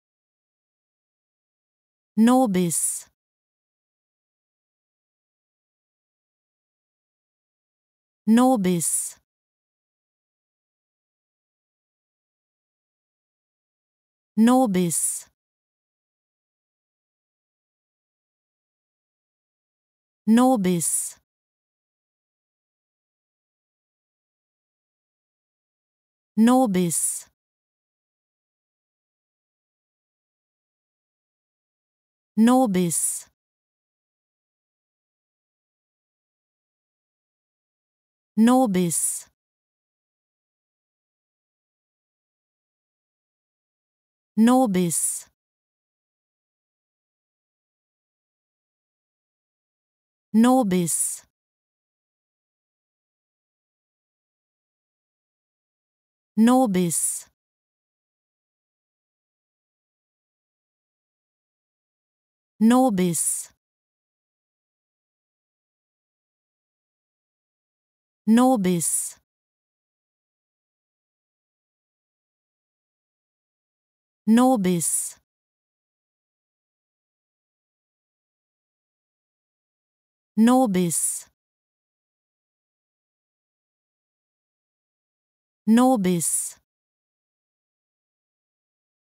How to Pronounce ''Nobis'' Correctly in German
Learn how to say ''Nobis'' in German with this short pronunciation tutorial.